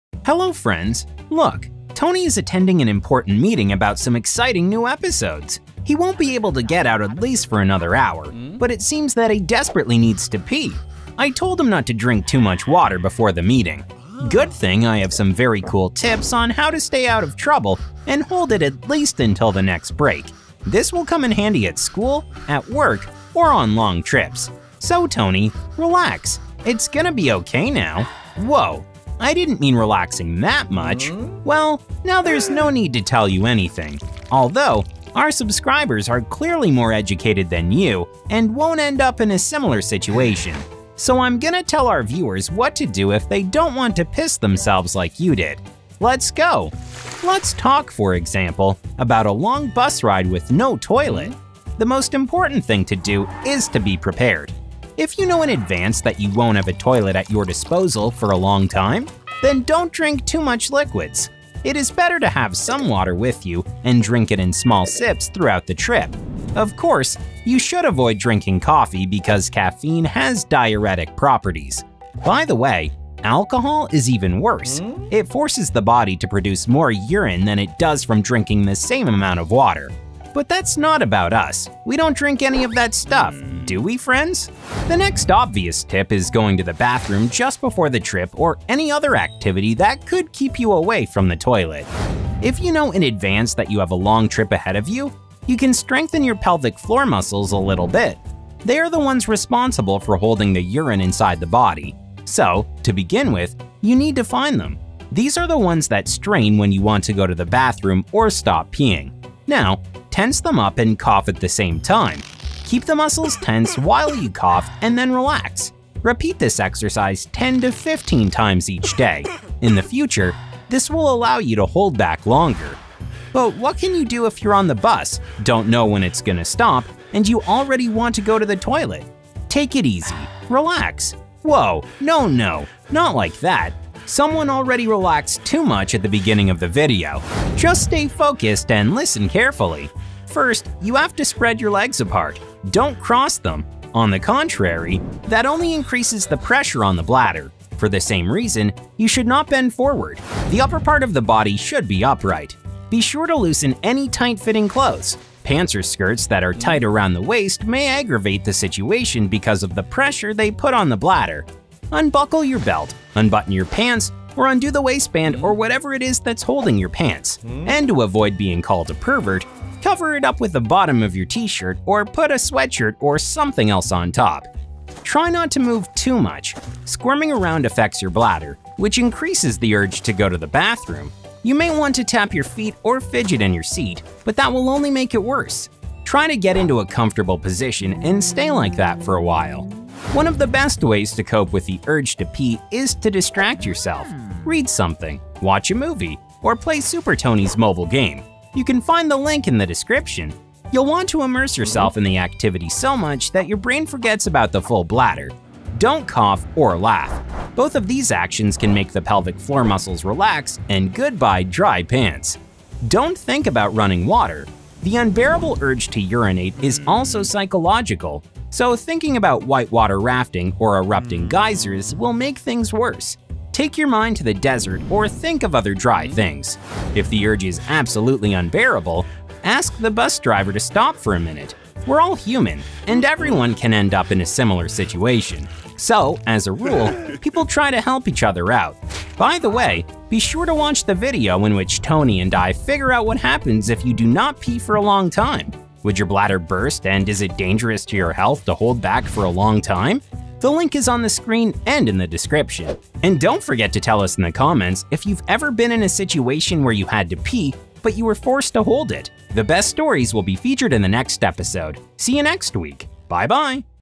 What if you want to pee, but you can't do it now
Hello! Tony here again got into an unpleasant situation, namely, he drank a lot of water before an important event, where there is no way to go to the toilet. As a result, he wanted to use the toilet at the most crucial moment. Therefore, today we will tell you what to do in order to endure as much as possible and not pee yourself. We think that this information will be very useful and will definitely come in handy in life. You won't find out such life hacks from Meet Arnold, believe me. Have a nice watching!